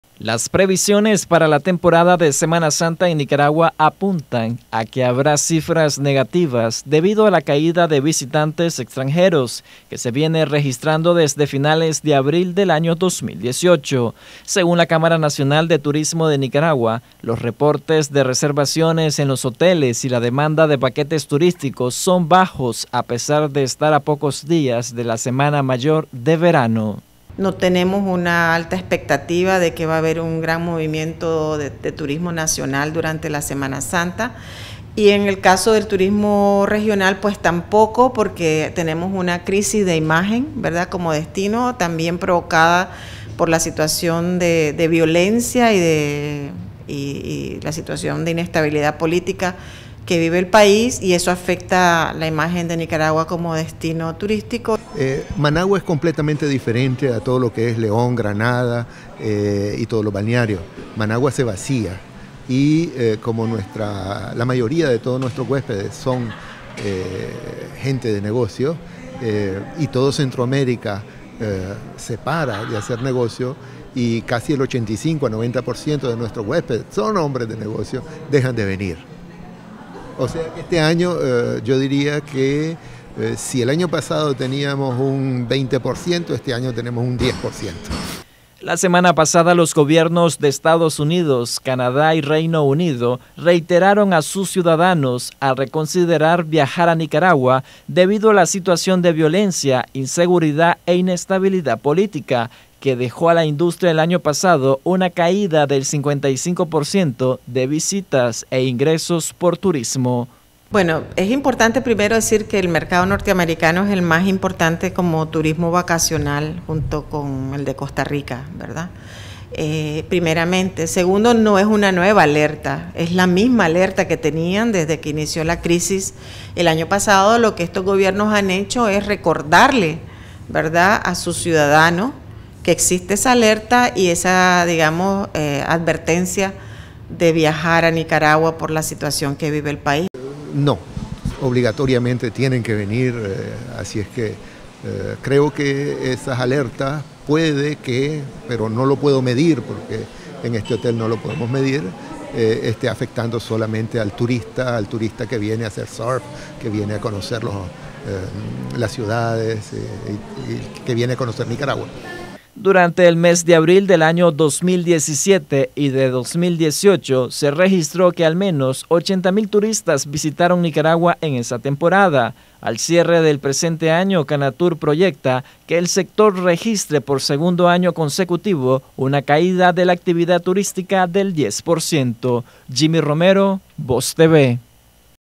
Turismo de Semana Santa sin metas de proyección por contexto sociopolítico en Nicaragua
Las previsiones para la temporada de Semana Santa en Nicaragua apuntan a que habrá cifras negativas debido a la caída de visitantes extranjeros que se viene registrando desde finales de abril de 2018.